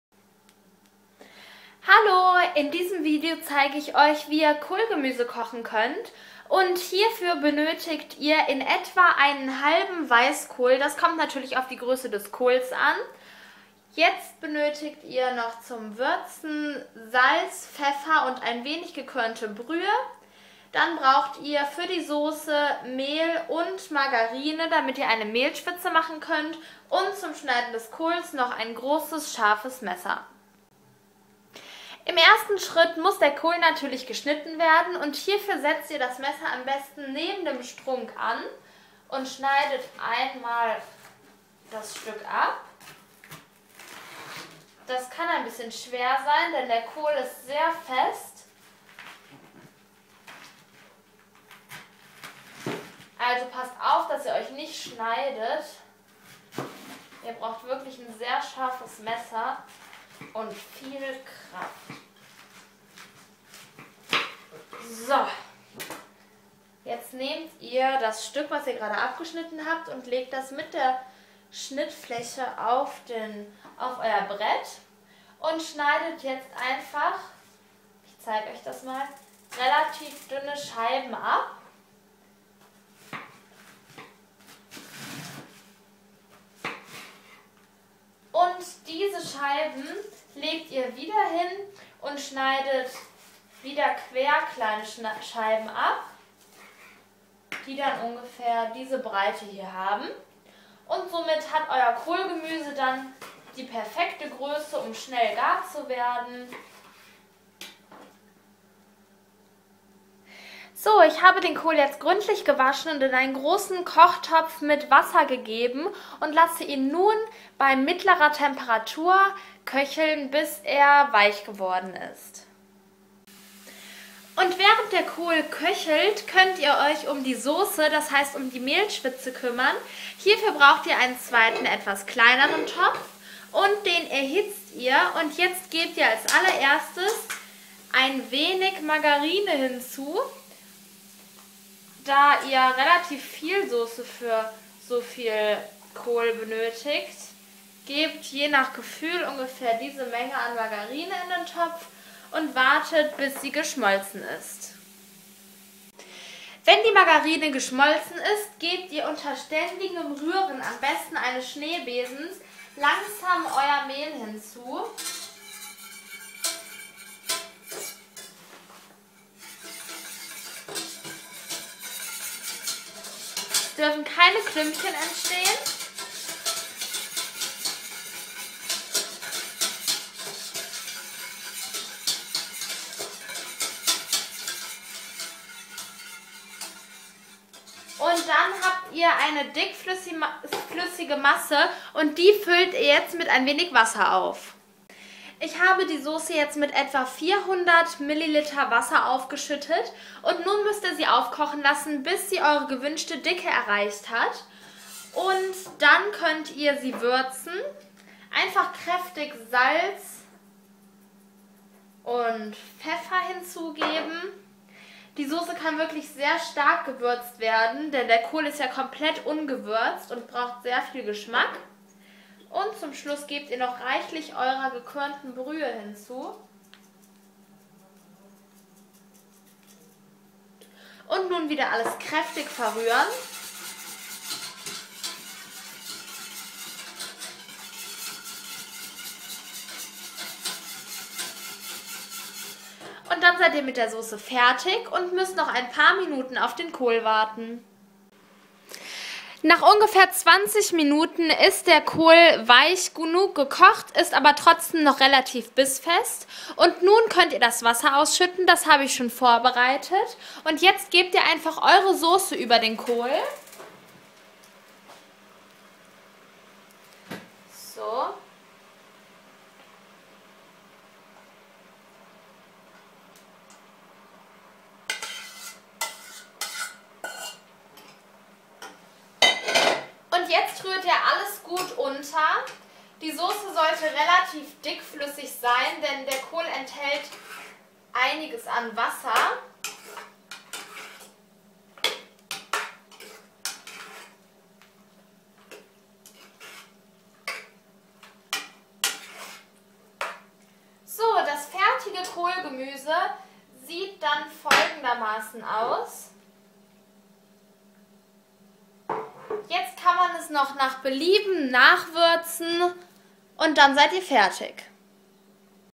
Kohlgemüse kochen - Rezept mit Sauce
Ihr könnt schnell Kohlgemüse kochen mit einer leckeren Sauce. Dazu braucht ihr Kohl, Mehl, Margarine, gekörnte Brühe und Gewürze. Zuerst wird das Gemüse gekocht und gleichzeitig rührt ihr die Soße an.

Weitere interessante Rezepte sowie Tipps und Tricks rund ums Essen und Trinken findet ihr in unserem Kanal